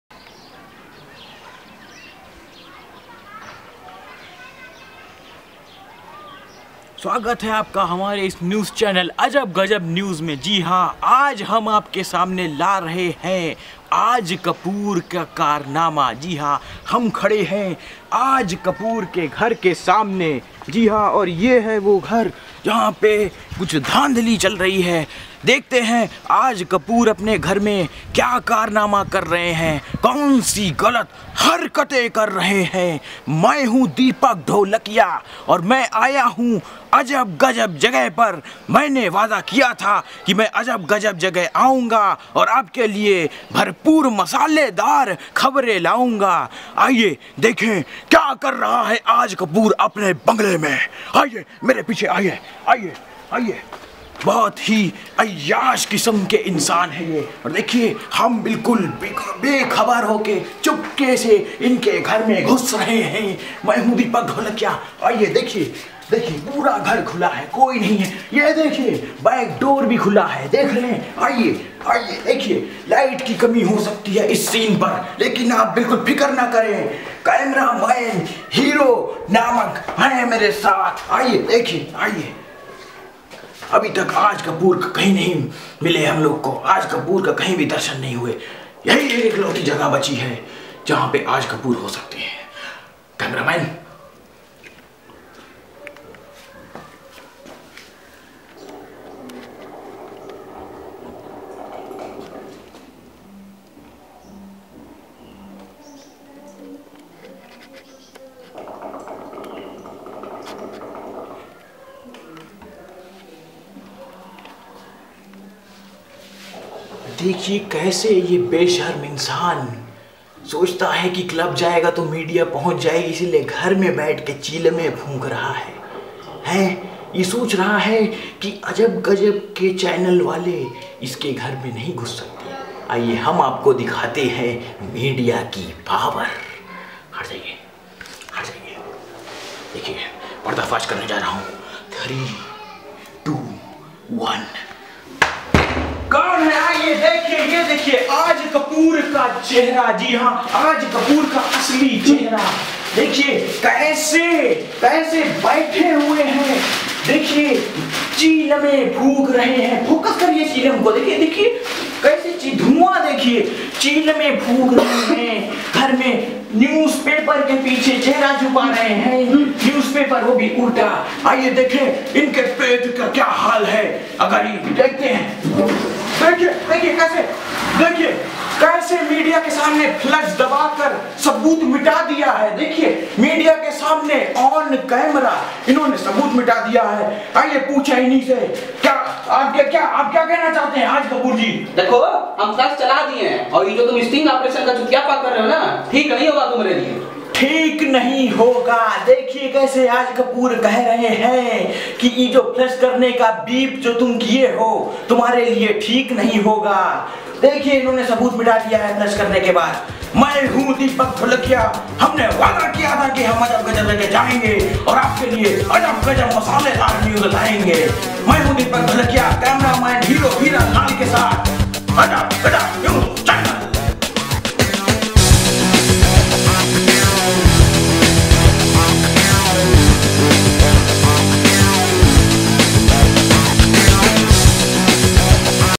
Ajab Gajab News Sting operation on Aaj kapoor
breathtaking sting video shows the actor Aaj kapoor and his mansion.